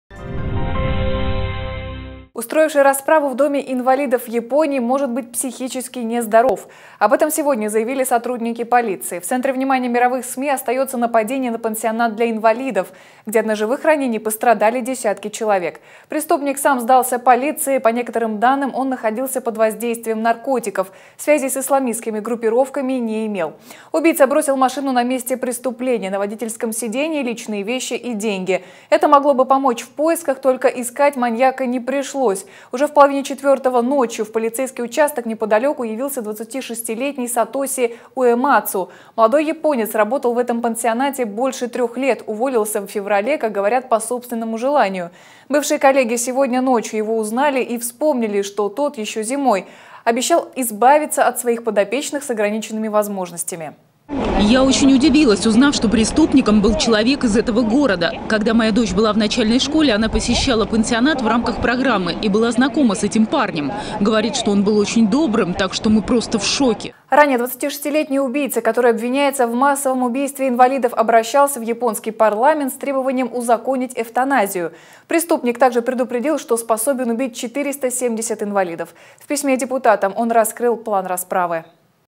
Расправа в доме инвалидов
Устроивший расправу в доме инвалидов в Японии может быть психически нездоров. Об этом сегодня заявили сотрудники полиции.  В центре внимания мировых СМИ остается нападение на пансионат для инвалидов, где от ножевых ранений пострадали десятки человек.